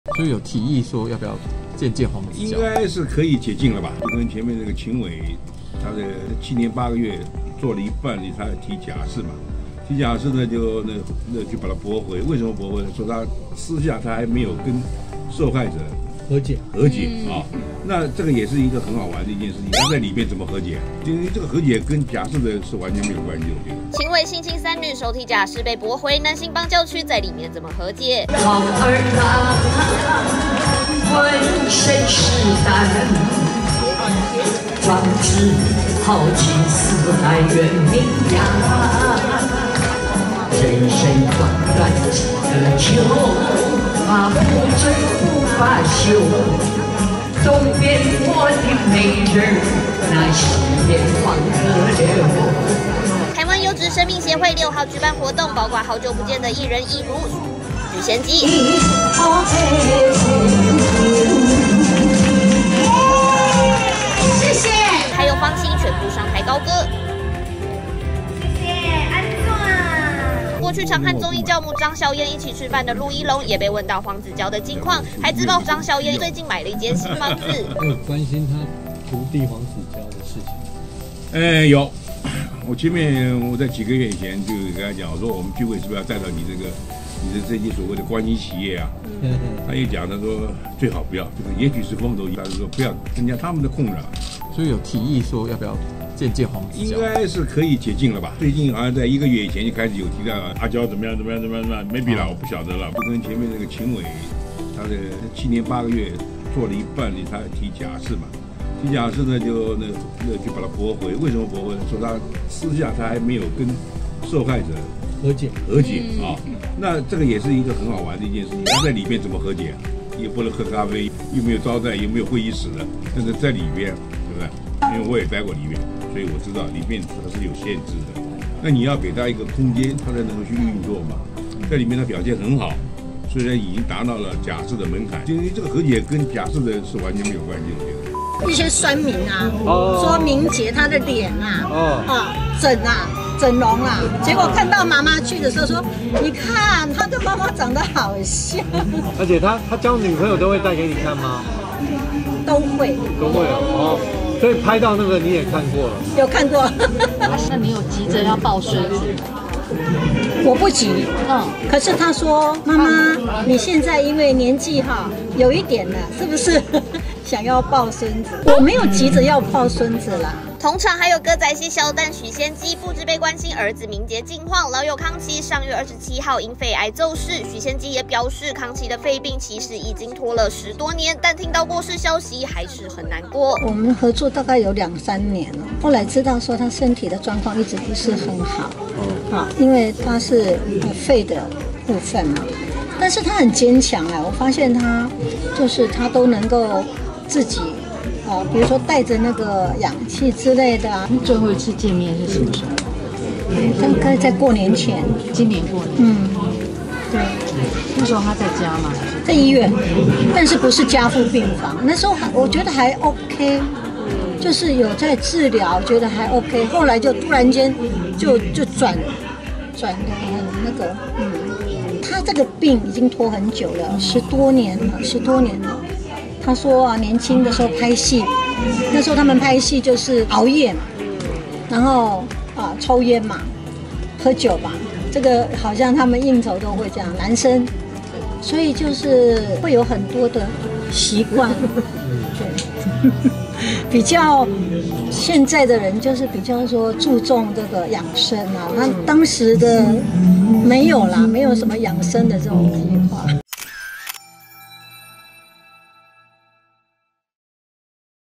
秦偉 性侵3女首提假釋被駁回 男星幫叫屈：在裡面怎麼和解｜三立娛樂星聞
★訂閱【女孩要幹嘛】甜寵浪漫戲劇看到飽